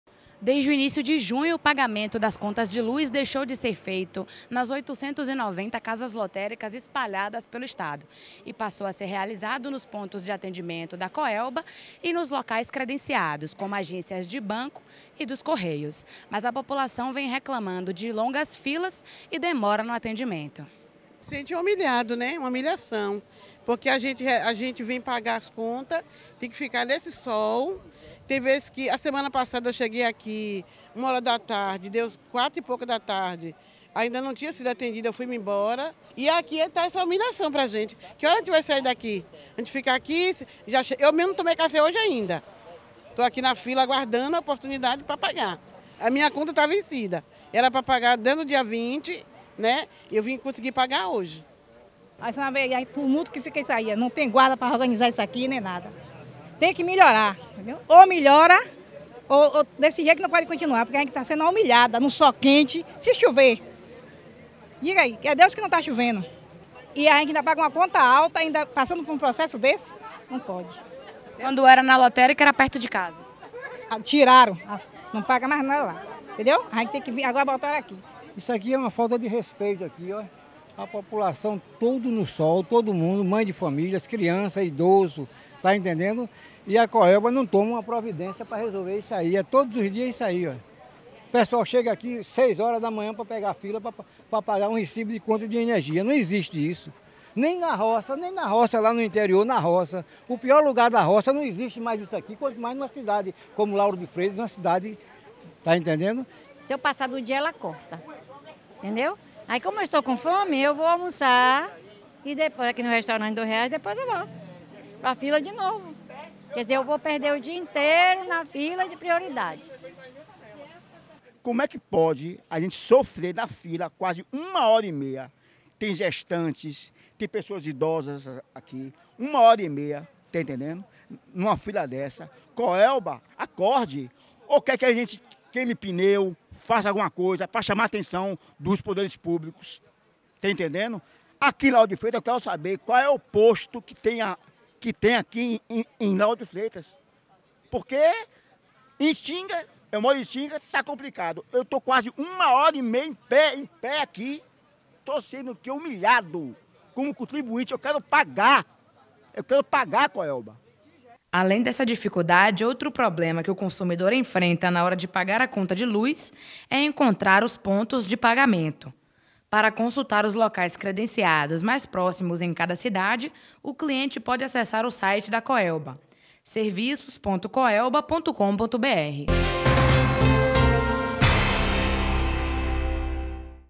Pagamento Coelba
Após o rompimento do contrato entre a Coelba e a Caixa econômica federal, pagar as contas de luz se tornou uma tarefa estressante para os consumidores.